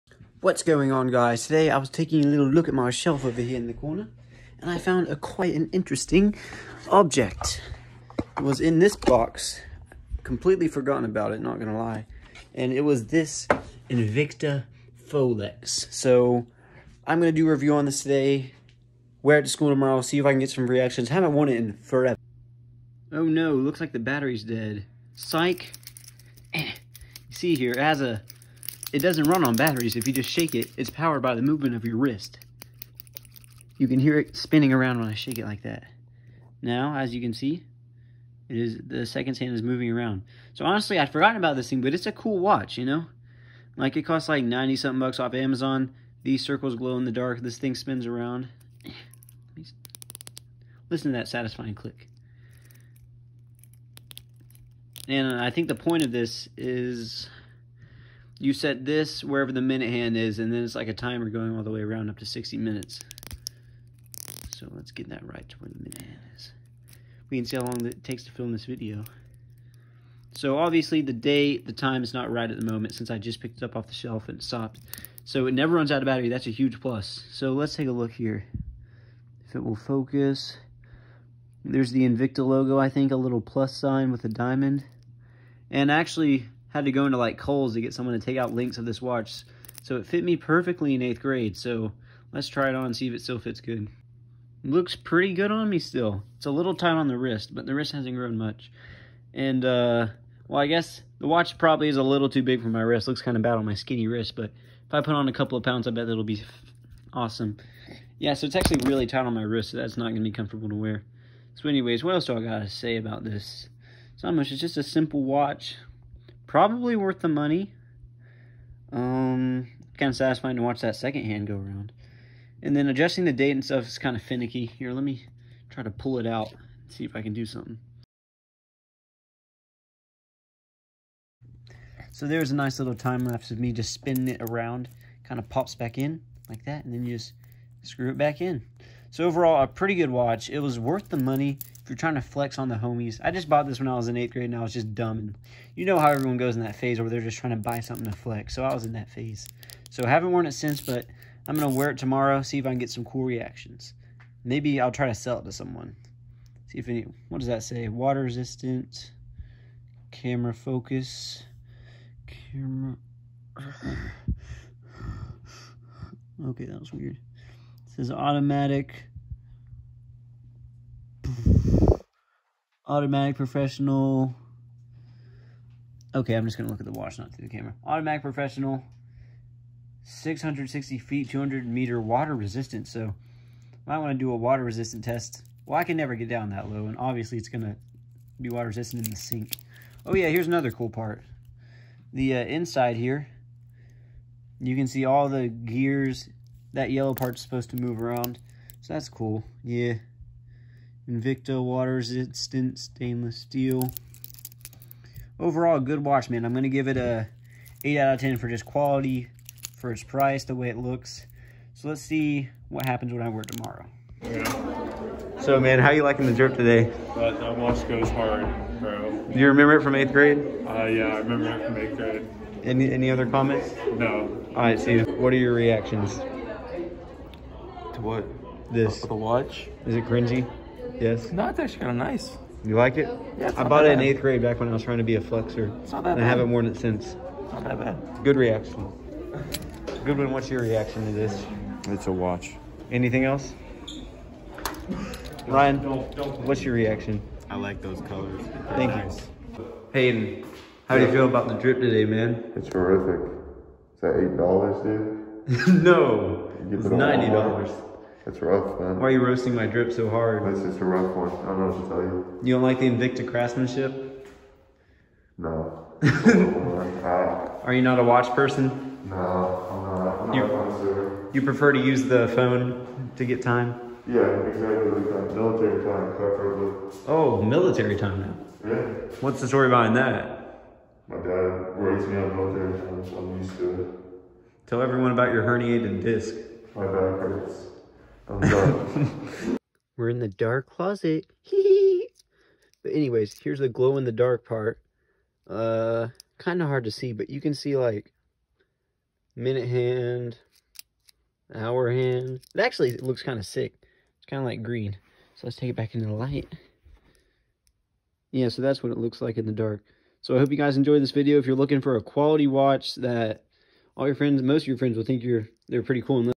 Fake Rolex review 💧 ❄️
Pretty drippy. I will sell it if your interested.